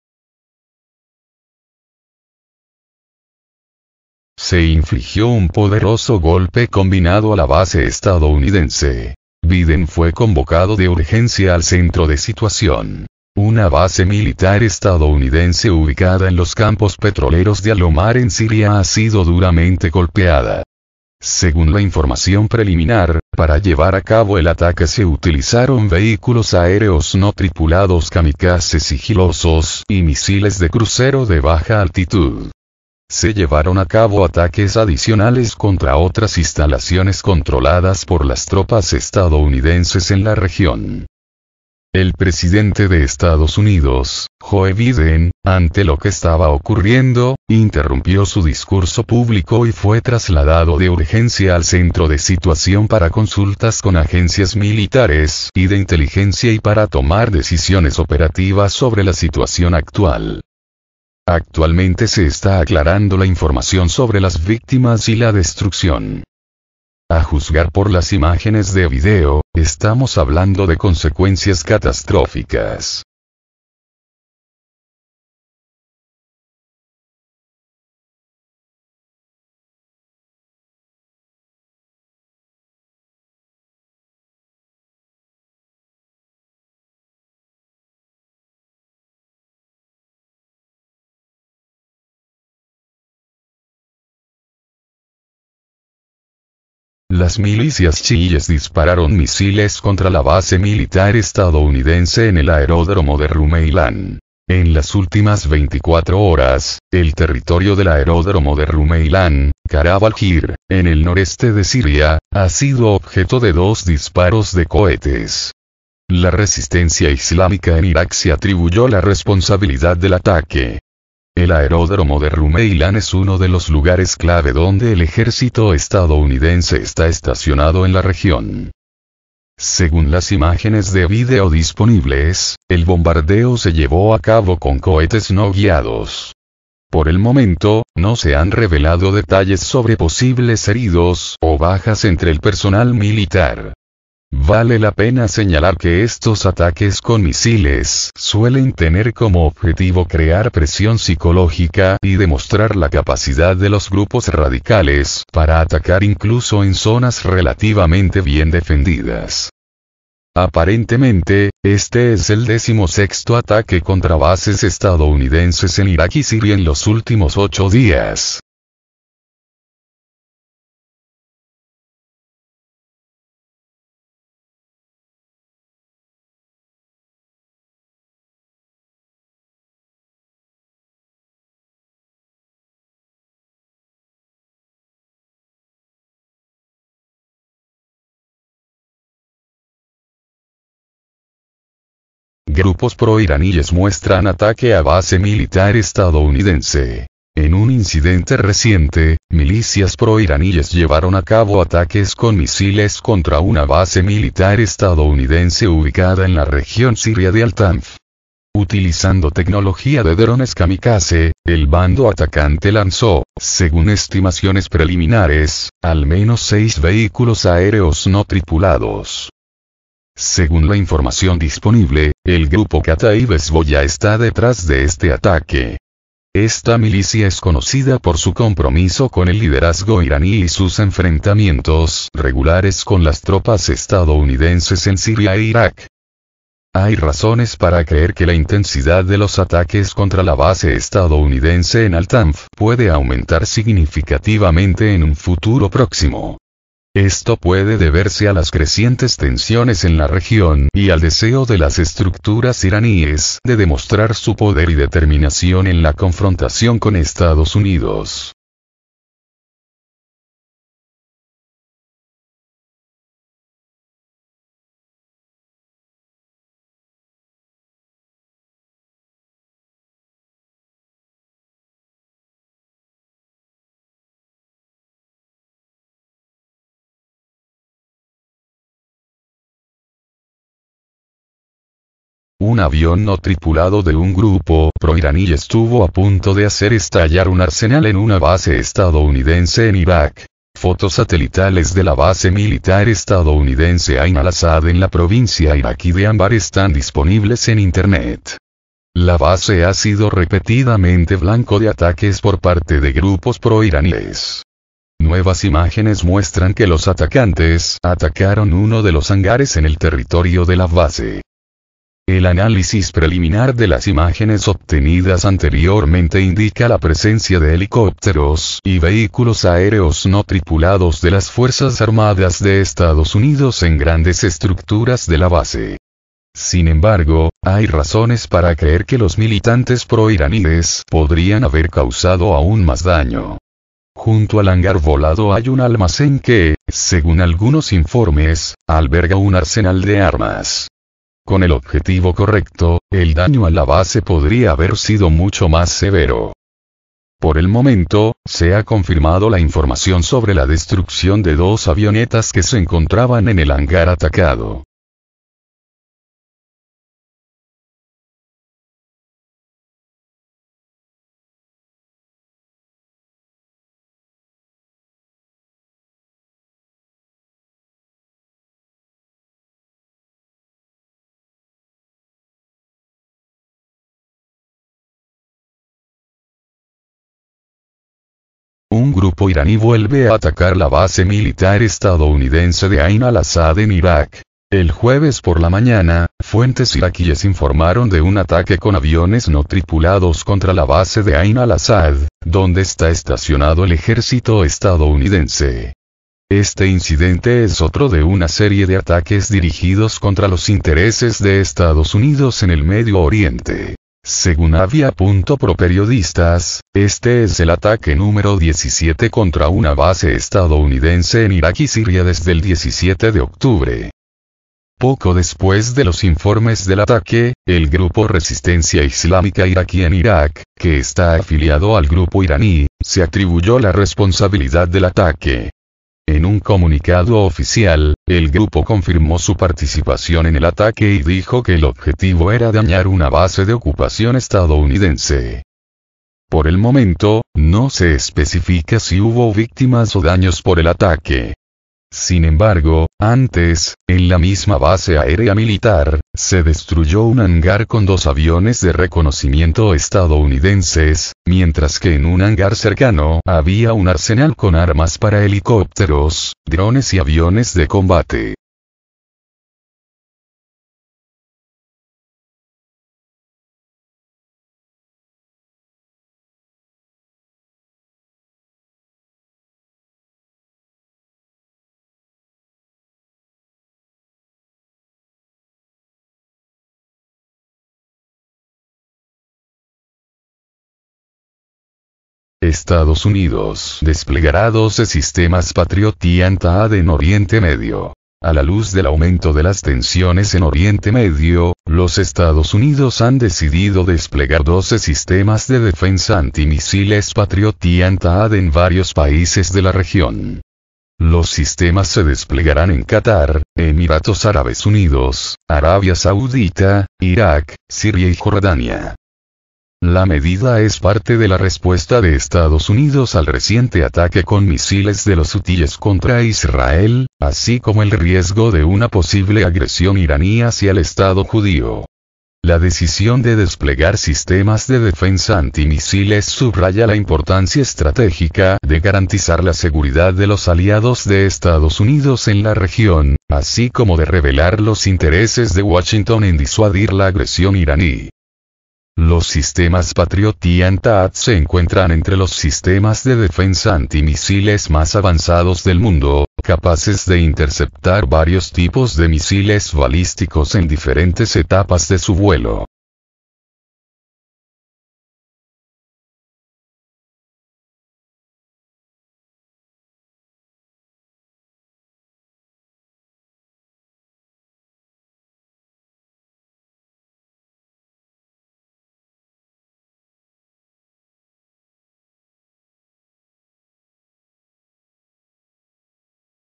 Poderoso golpe combinado a base de EEUU: Biden fue convocado de urgencia al Centro de Situación
Las milicias chiíes dispararon misiles contra la base militar estadounidense en el aeródromo de Rumeilan.

Grupos proiraníes muestran ataque a base militar estadounidense.

Un avión no tripulado de un grupo proiraní estuvo a punto de hacer estallar un arsenal en una base estadounidense en Irak.

Un grupo iraní vuelve a atacar la base militar estadounidense de Ain al-Assad en Irak.

EE.UU. desplegará 12 sistemas Patriot y THAAD en Oriente Medio